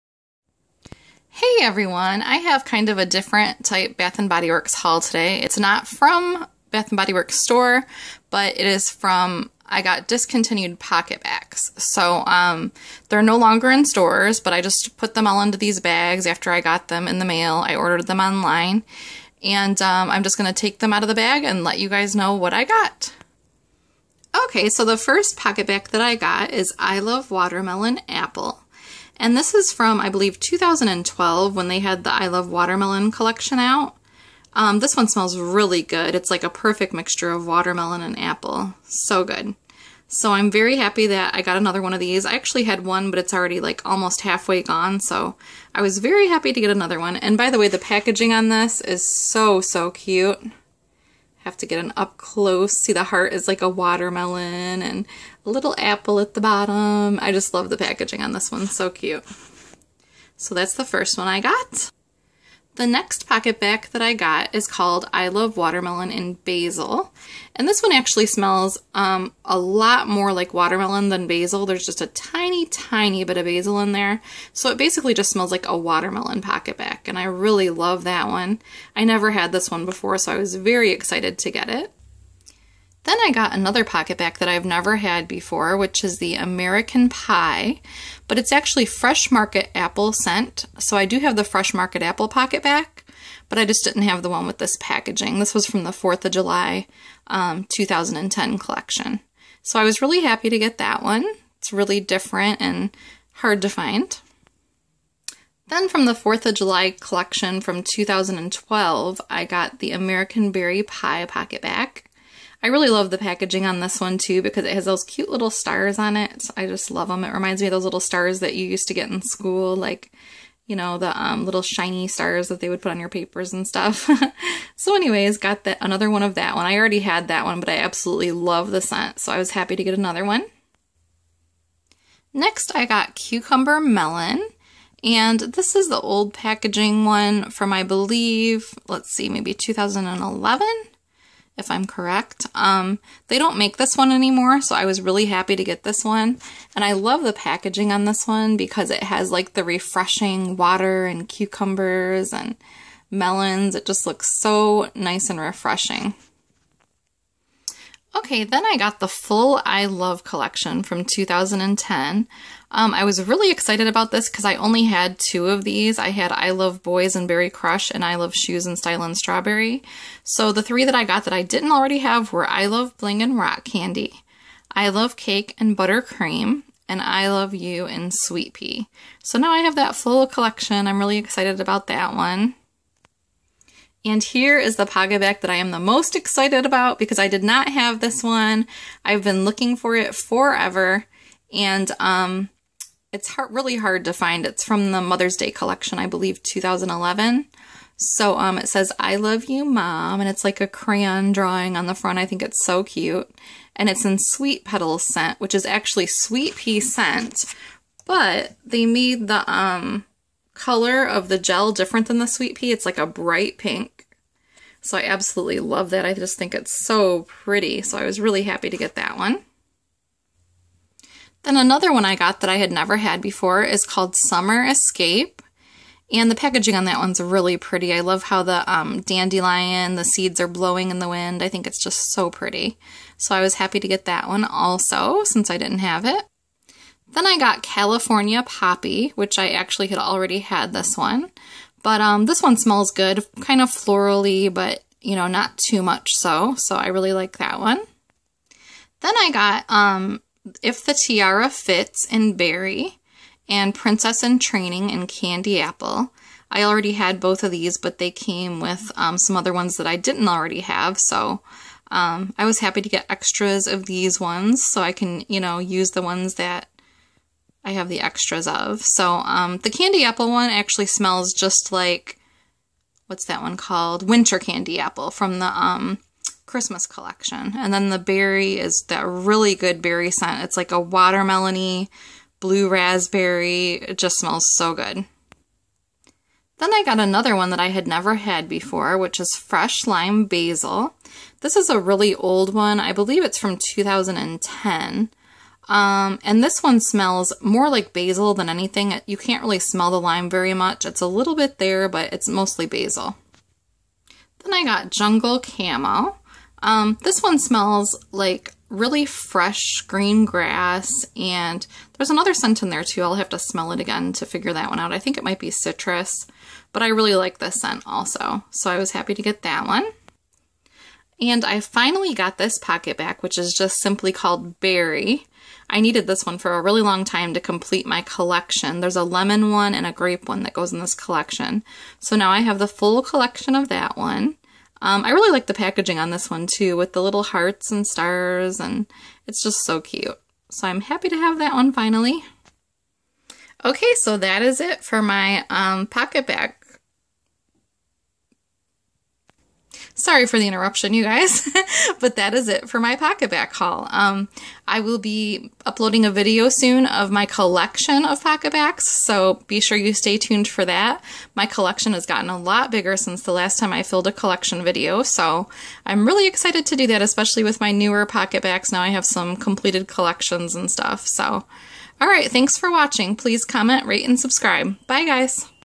Huge Bath & Body Works Discontinued Pocketbac Haul!!
Hey guys! I've been saving up the discontinued pocketbacs I've been getting in the mail lately to make you this collective haul video! I hope you enjoy watching!